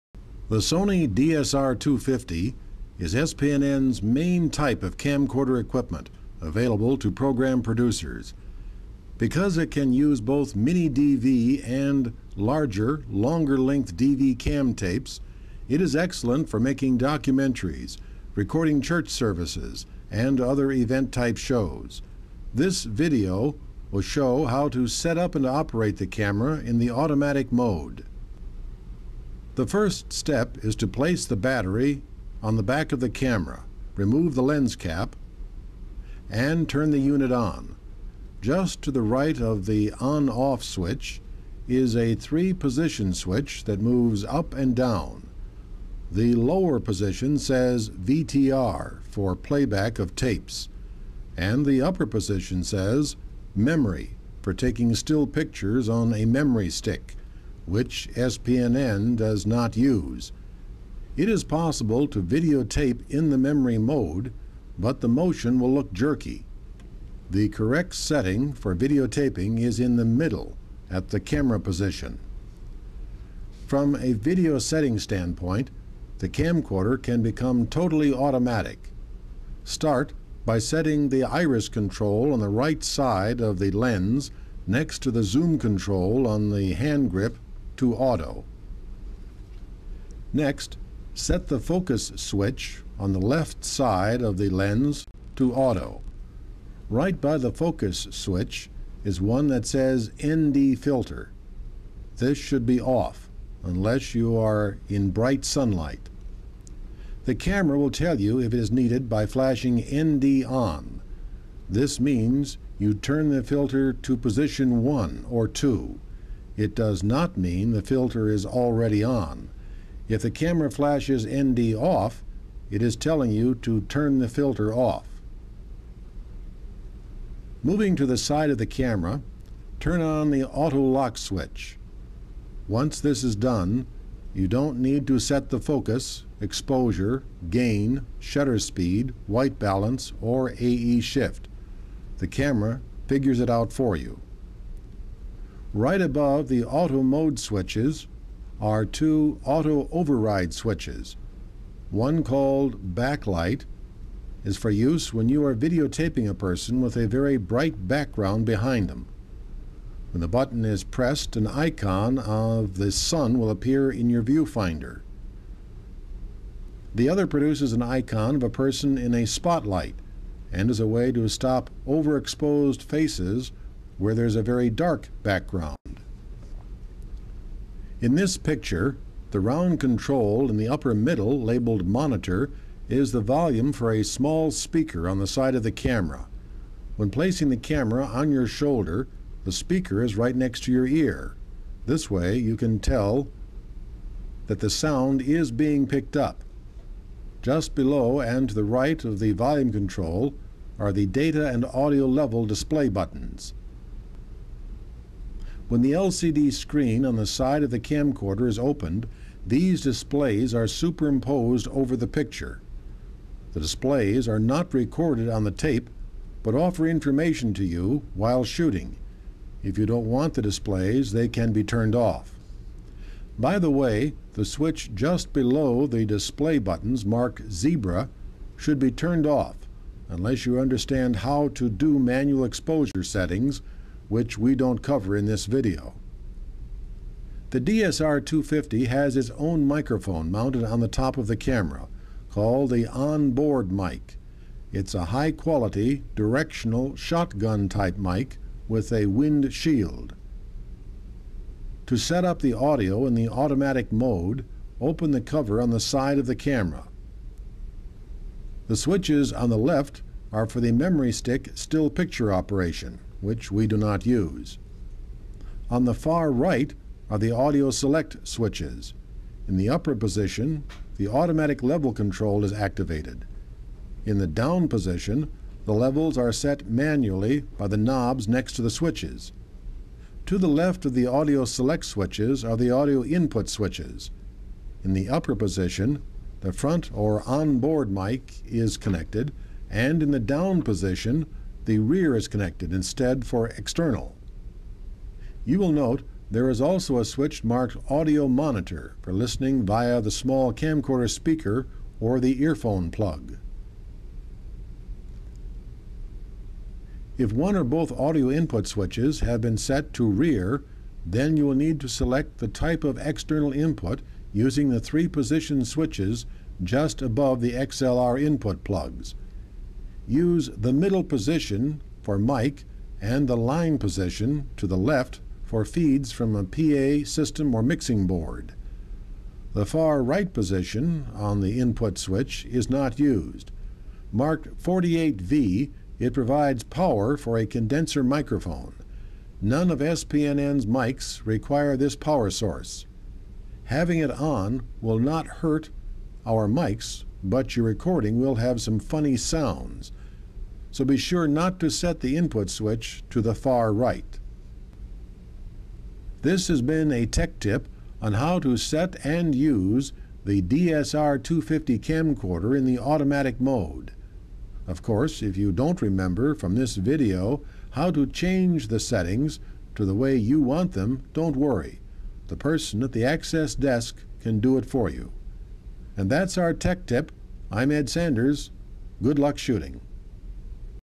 [SPNN] DSR 250
A indepth tutorial on the Sony DSR 250 provided by SPNN Access Staff